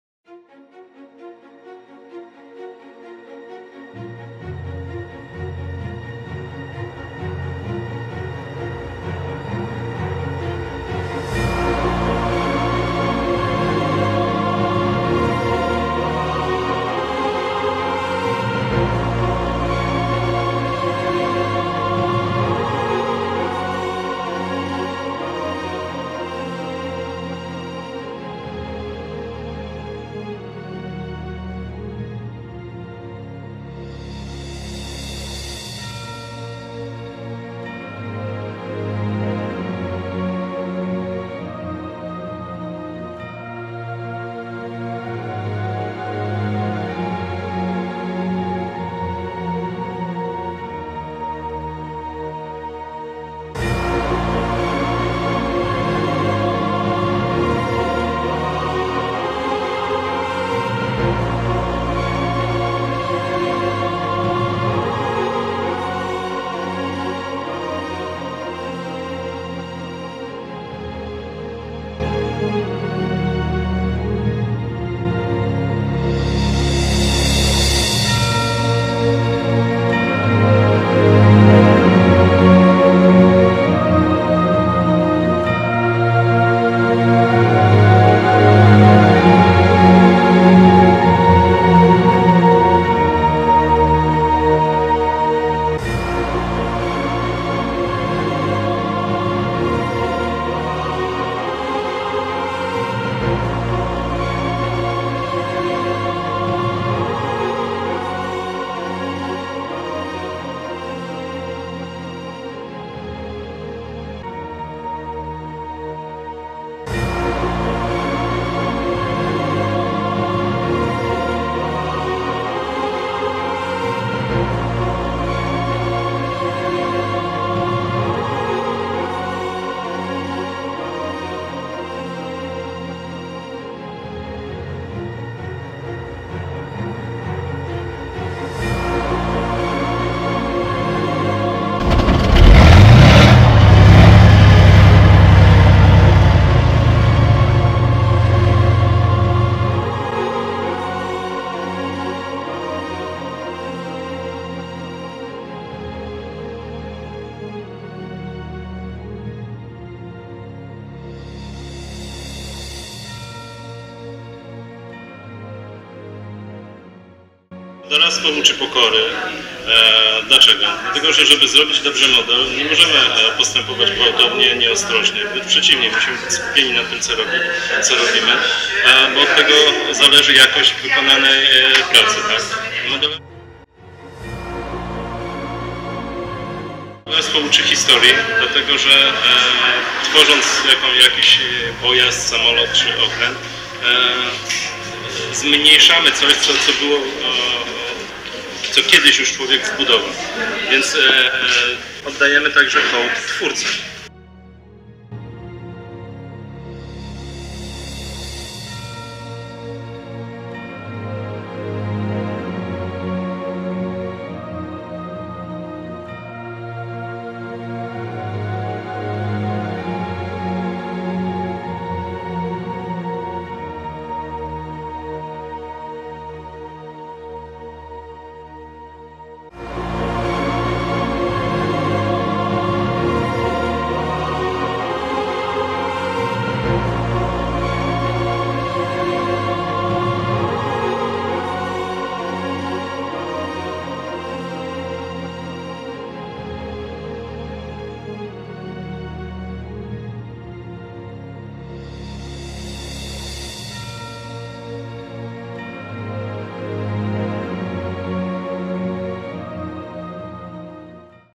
II WŁOCŁAWSKI FESTIWAL MODELARSKI 2018
Projekt gimnazjalny II włocławski festiwal modelarski.
Opiekun projektu: Paweł Popławski - nauczyciel fizyki.